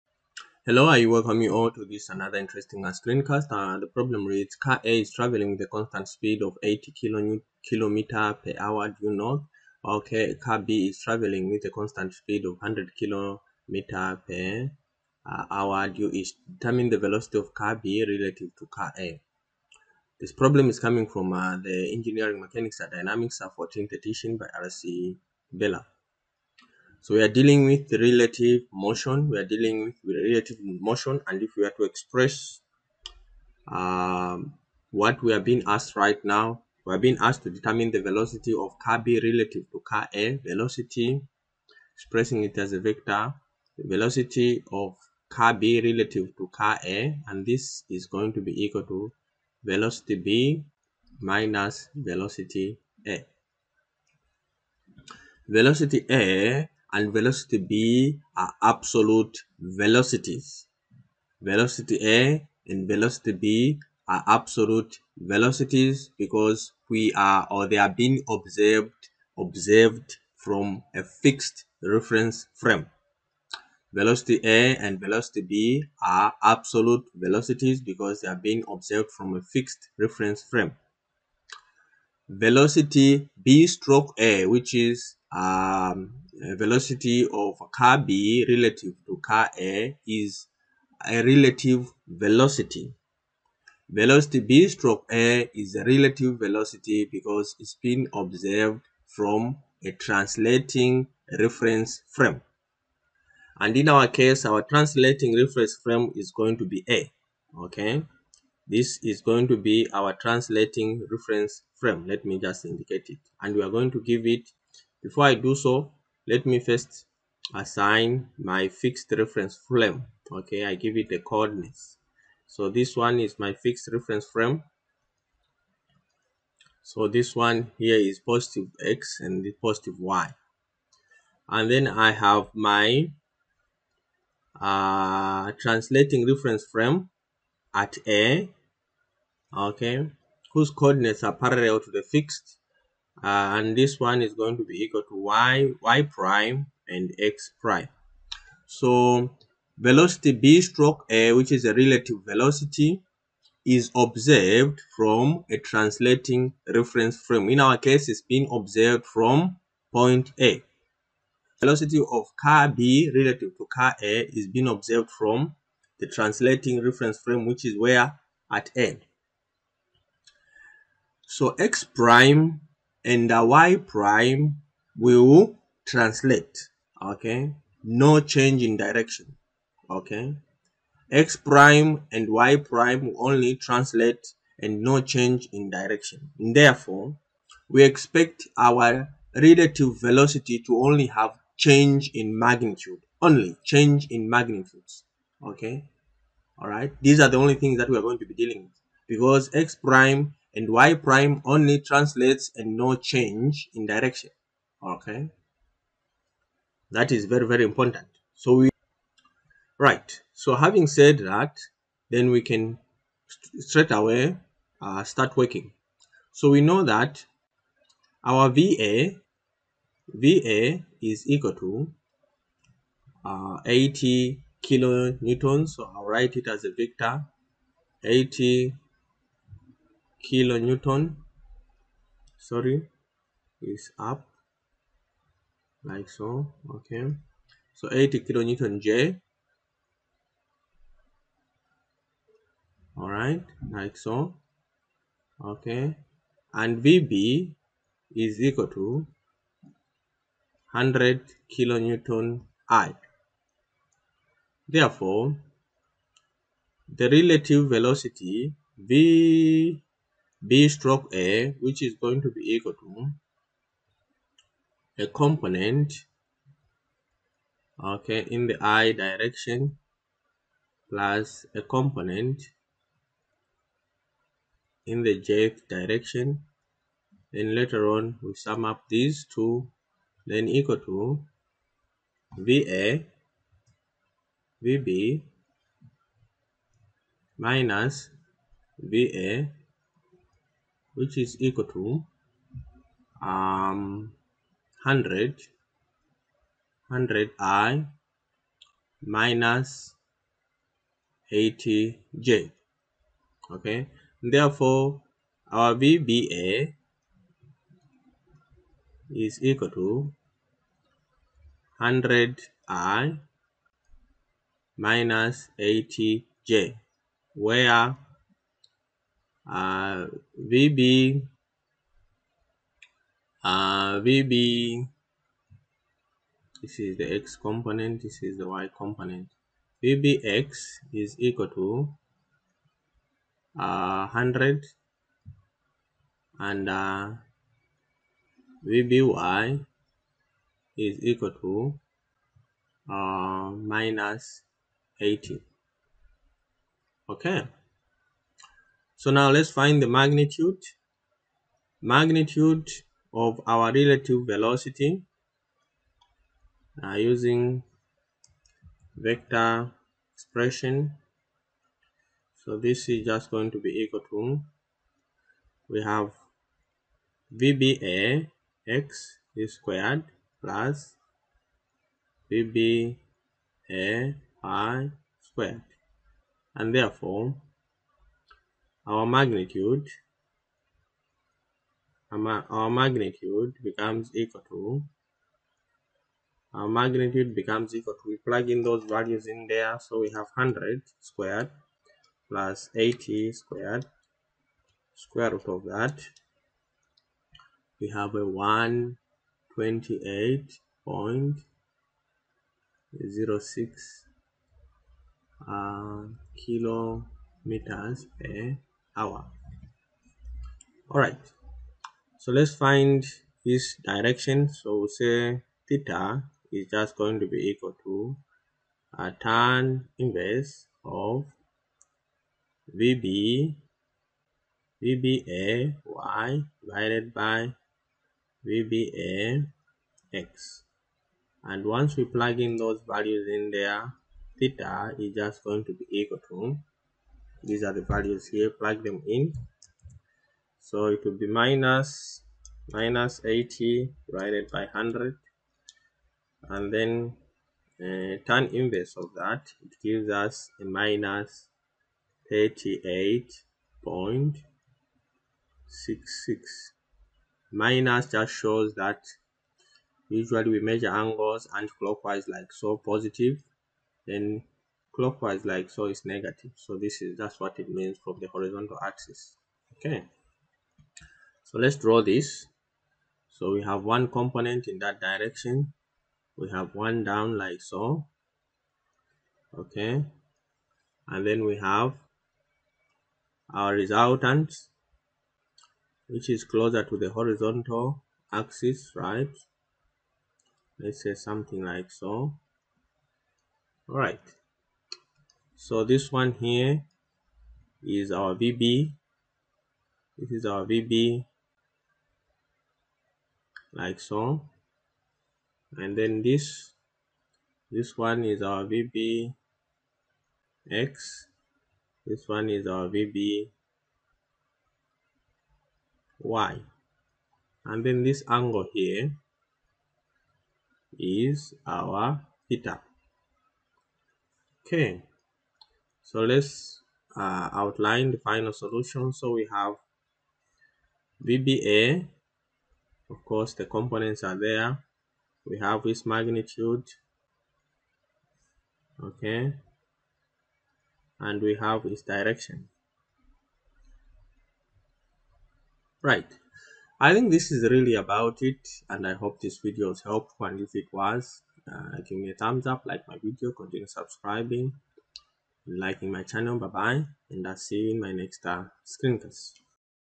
F12–45 Kinematics of a Particle (Chapter 12: Hibbeler Dynamics) Benam Academy
ENGINEERING MECHANICS - DYNAMICS, 14TH EDITION, R. C. HIBBELER
CHAPTER 12: Kinematics of a Particle
PROBLEM: F12–45
F12–45. Car A is traveling with a constant speed of 80 km/h due north, while car B is traveling with a constant speed of 100 km/h due east. Determine the velocity of car B relative to car A.